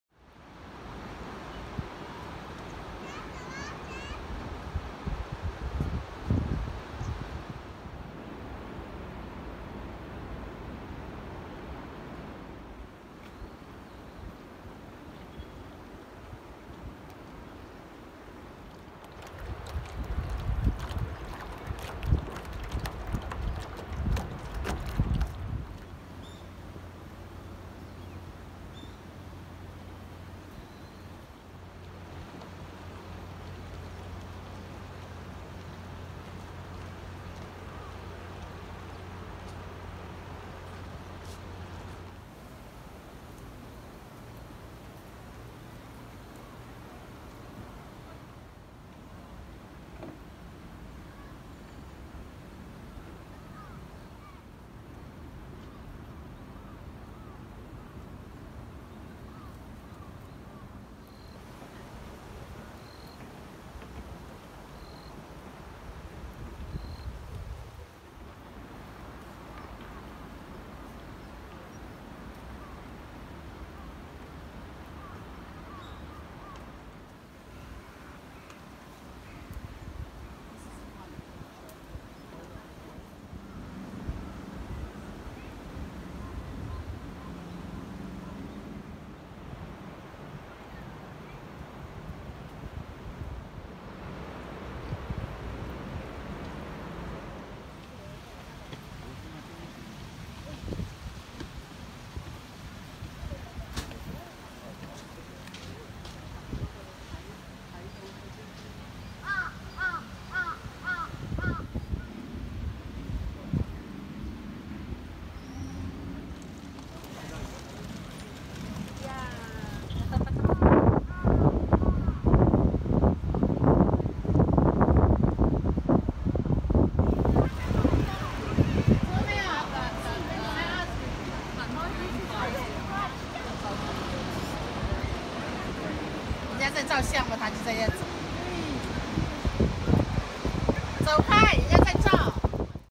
【秋の京都嵐山】 嵐山の紅葉状況🍁2019年11月4日(月) 🌟Autumn in Arashiyama✨ 岚山【4K】
AM9:30頃 今年の嵐山の紅葉は現在こんな感じです。
この辺りは景色が良いのに人が少ないのでおススメですよ！